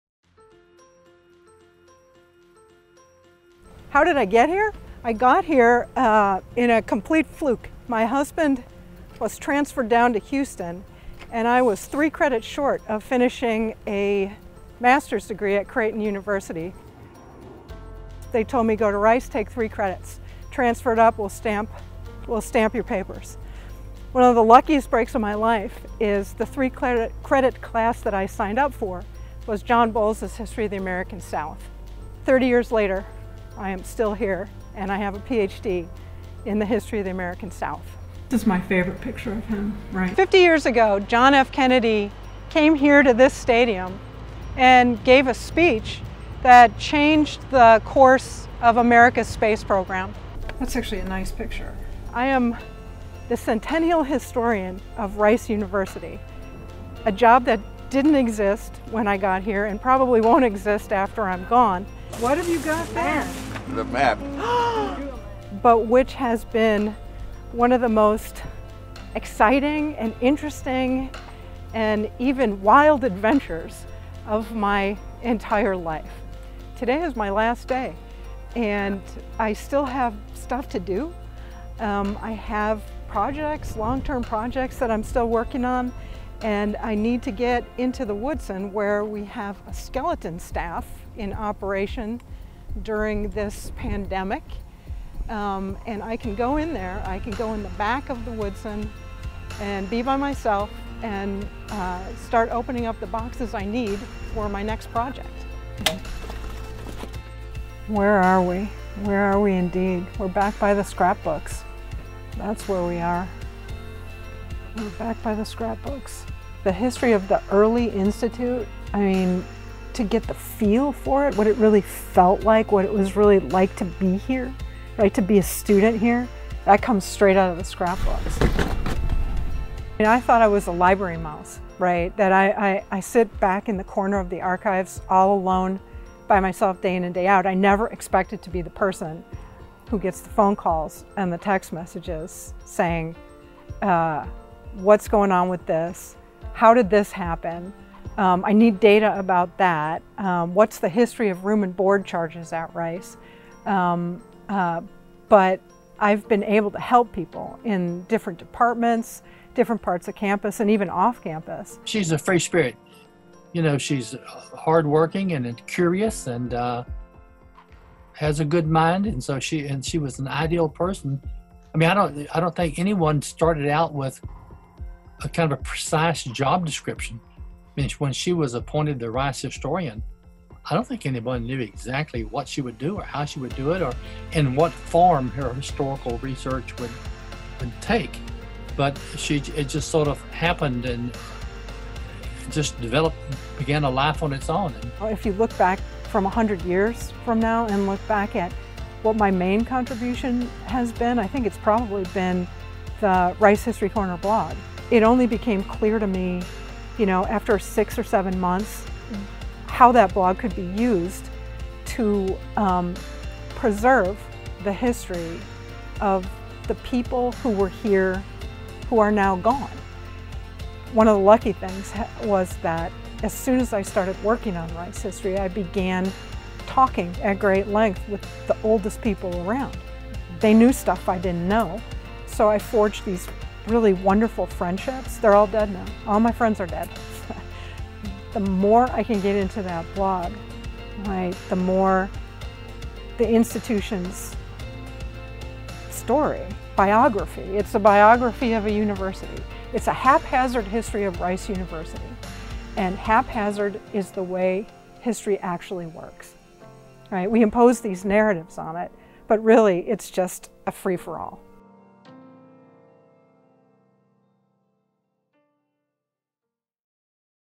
Rice historian retires after 30 years at Rice
Until last week, Melissa Kean was the Centennial Historian of Rice University.
 It was, in her words, “a job that didn't exist when I got here and probably won't exist after I'm gone, but which has been one of the most exciting and interesting and even wild adventures of my entire life.”
 Over the past three decades, Kean carved out a niche as the go-to vessel of institutional knowledge about a university she’d only come to by chance. Kean was three credit hours shy of her master’s degree at Creighton University in Omaha, Nebraska, when her husband was unexpectedly transferred to Houston in 1991.
 “They told me, 'Go to Rice, take three credits, transfer it up and we'll stamp the papers,” Kean said of Creighton. “One of the luckiest breaks in my life is the three-credit class that I signed up for was John Boles’ History of the American South.”
 Kean would eventually earn her Ph.D. from Rice in that very same area, studying under famed history professor Boles. She became the university’s official historian in 2005 and spent her first several years hunting down and “bringing in the stuff that people were squirreling away,” from an “Owl-opoly” board game to the spectacles belonging to Rice’s first president, Edgar Odell Lovett.
A former lawyer, Kean soon combined her knack for writing with the knowledge gleaned during her doctoral work and a relentless amount of research in Rice’s vast archives to produce the popular Rice History Corner blog. She announced her retirement there in a June 29 post that also introduced her new grandson.
For over a decade, Kean’s blog showcased “newspaper articles, scrapbooks, photographs, letters from homesick freshmen, diaries, memos from presidents and provosts, minutes of department meetings, bits and scraps found in every corner of the campus and beyond” in daily updates. It quickly grew into a popular virtual meeting space for Rice alumni and history buffs alike, amassing thousands of regular readers.
In 2012, her research was vital to Rice’s long-awaited centennial celebration. Kean herself appeared in dozens of Centennial Series videos detailing everything from the history of O-Week to the university’s oft-forgotten first student center.
Along with Rice colleague Joshua Furman, Kean was instrumental in helping salvage documents from synagogues flooded during Hurricane Harvey in 2017; much of what they saved would later be preserved in the Houston Jewish History Archive, established in 2018 at the Woodson Research Center inside Fondren Library.
 Kean often spent up to 10 hours per day inside the Woodson archives, emerging with exciting new images or documents to share with Rice History Corner readers: candid shots of Black student life in 1994 or a rare shot of Lovett smiling.
 Over time, Kean became known as the person to ask about anything related to Rice that happened in the past — and Kean herself, recognizing her responsibility as custodian of Rice’s history, turned to those who came before her in an effort to preserve their memories of the institution.
 “One of the lucky things was that as soon as I started working on Rice history, I began talking at great length with the oldest people around,” Kean said. “They knew stuff I didn't know.
“They're all dead now,” she continued. “And no one else is ever going to be able to intentionally talk to those people the way that I did.”
Kean’s mentor and former professor, Boles, retired from teaching at Rice last year and bid farewell in a massive reception at the Moody Center for the Arts. Neither he nor Kean could have predicted her career when she first enrolled in his History of the American South course in the early 1990s.
“Historians tend to depend upon documents and written sources to tell the history,” said Boles, who also directed Kean’s dissertation. “But all of us know the written documents don't tell the entire history — you need to talk to people, and you often can find out things verbally or orally that you cannot find written.”
This, Boles said, is what set Kean apart.
“She identified people who had been important at Rice or knew a lot about Rice, and she liked them and they liked her, so she talked and they told her things,” he said of Kean’s work, which also often resulted in gathering even more materials from those people to bring back to the Woodson Research Center.
“In some sense, she began an unofficial oral history of Rice,” Boles said. “She did that for years with great scale and the result is thousands of these little historical nuggets.”
Boles likened Kean to an archaeologist, “digging up the bones of the past.” It was a job she performed “with great dedication and great joy,” Boles said.